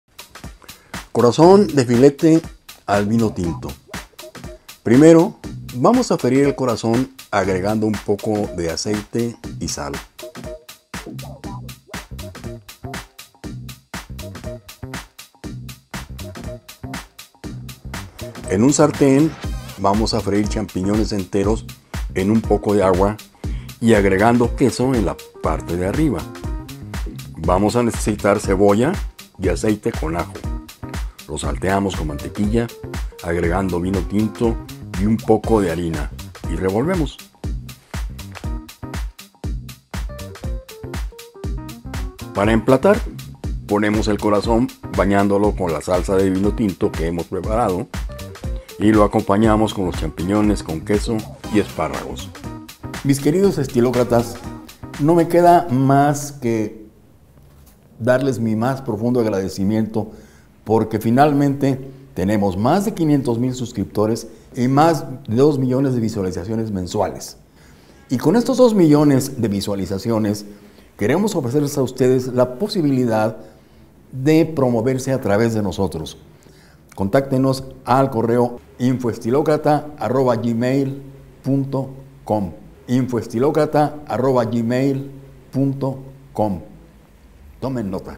CÓMO HACER SALSA DE VINO TINTO CON FILETE DE RES: UNA RECETA FÁCIL GOURMET Y DELICIOSA
ESTA RECETA COMPARTIDA POR EL RESTAURANTE TOSCALIA, NO TE LA PUEDES PERDER. ES MUY FÁCIL DE HACER Y DELICIOSA. SORPERNDE A TUS INVITADOS.

Visita nuestro sitio asociado de Amazon para productos de tu interés: